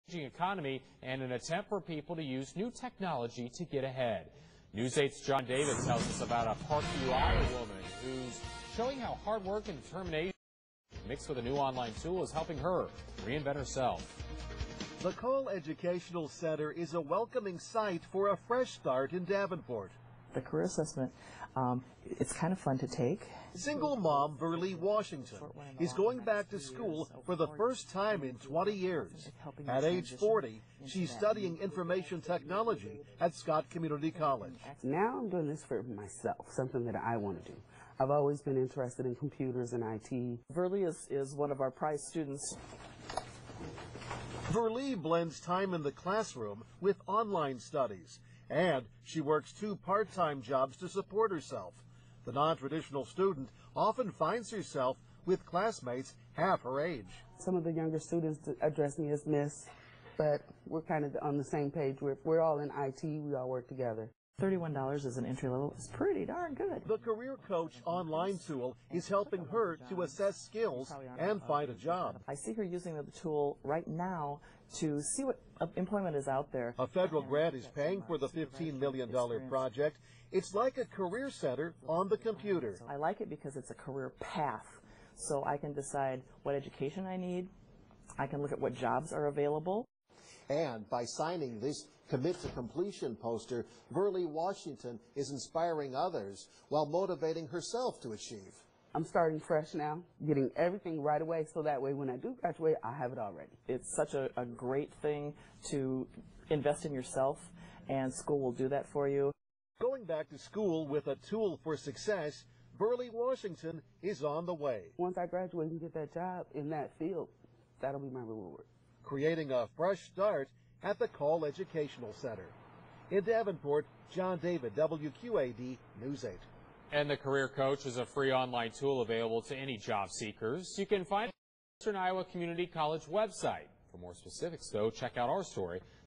EICC Career Coach online tool helps Park View woman to build a career
Thanks to WQAD-TV for the video.

Career Coach, a free online tool, allows anyone to search for potential careers. It provides a wealth of resources and is tailored to the Eastern Iowa and Western Illinois area.

Those using it will find information about the jobs currently available locally, wage projections, current employment numbers, who’s hiring, the approaching retirement age of current workers and more.

The site is available by going to the EICC and clicking on the Career Coach link at the bottom of the page or directly